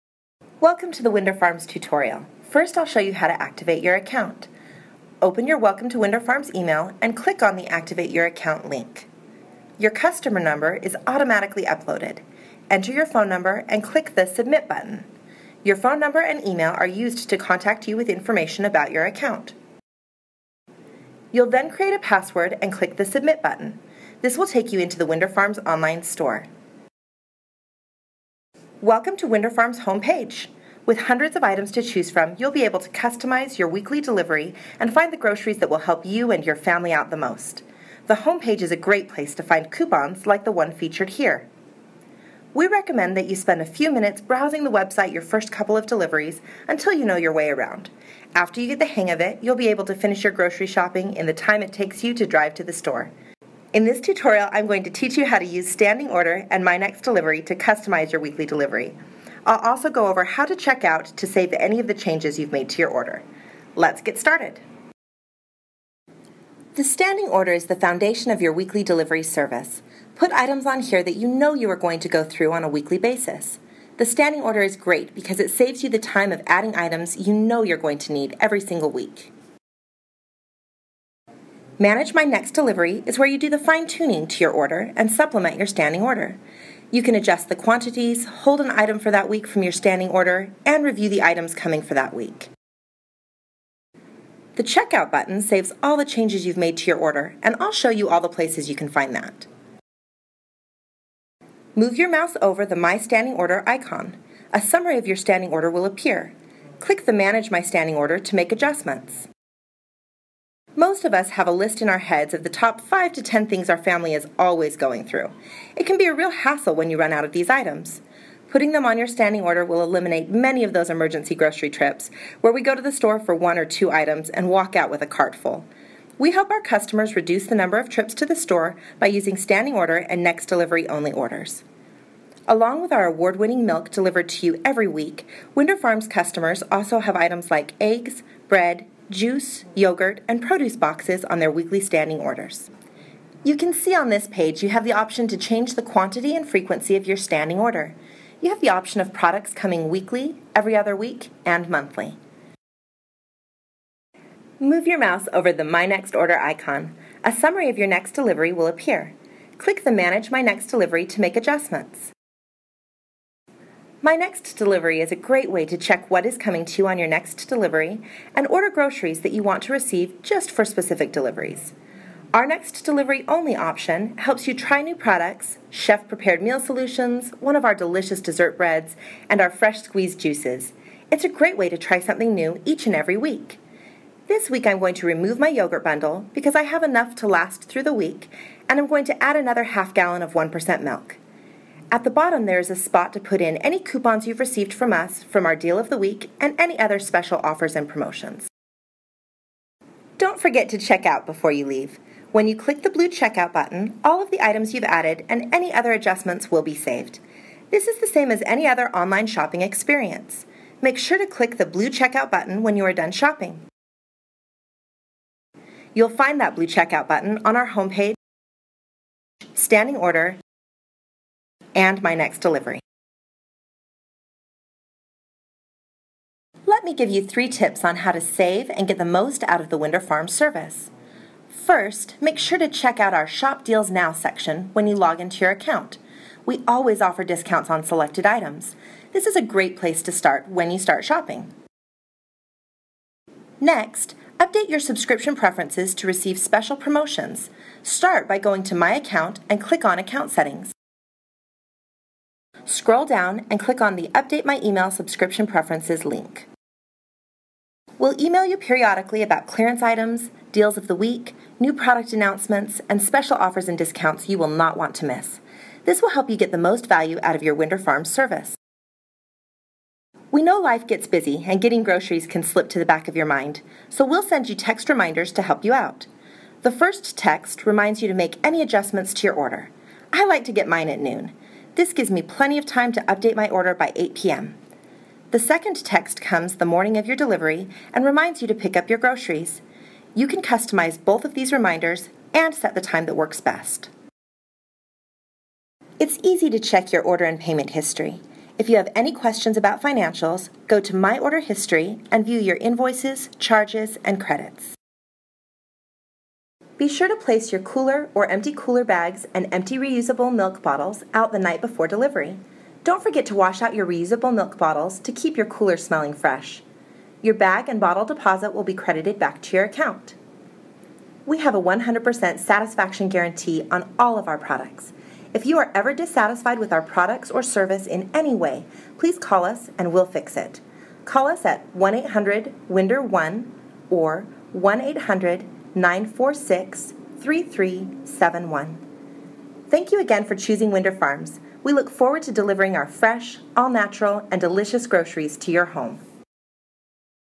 Winder Farms Tutorial
Here is a tutorial to help you get the most out of your Winder Farms service.